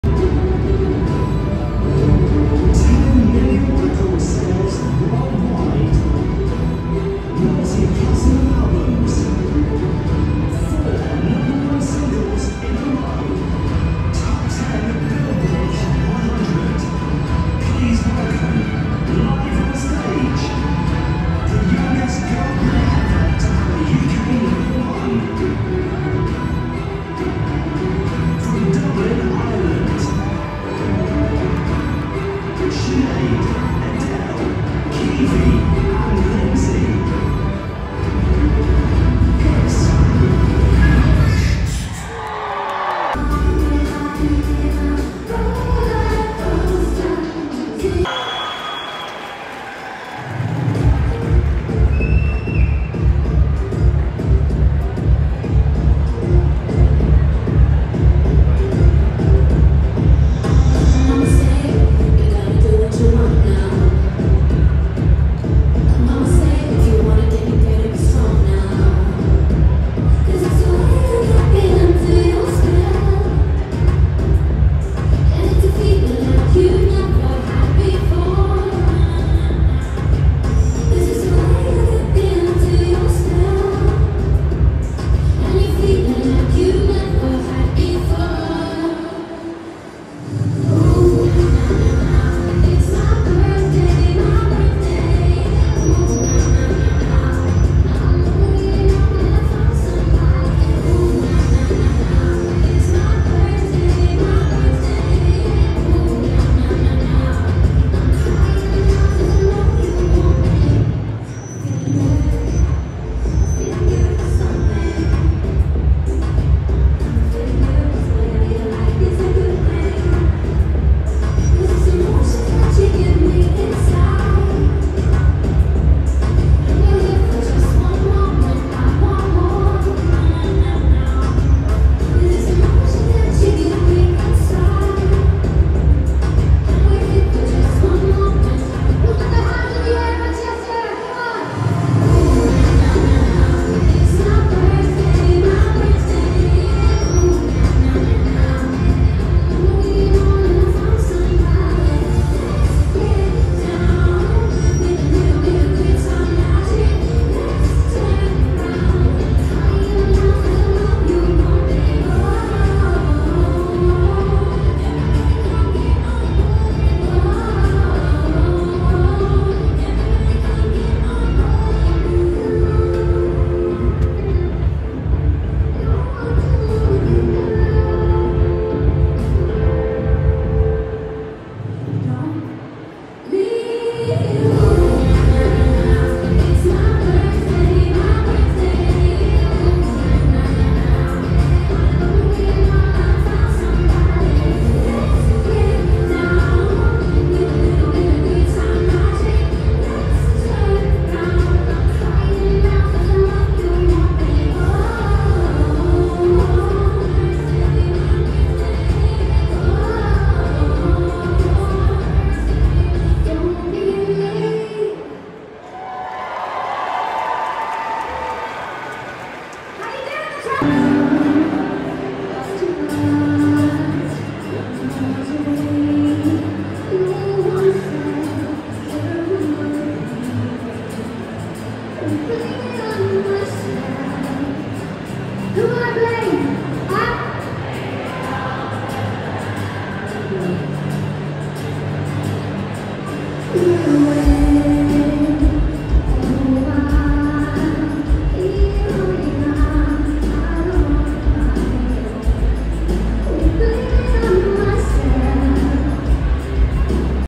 B*WITCHED performs in Manchester
90’s Top B*witched performance in Manchester at Trafford centre anniversary celebrations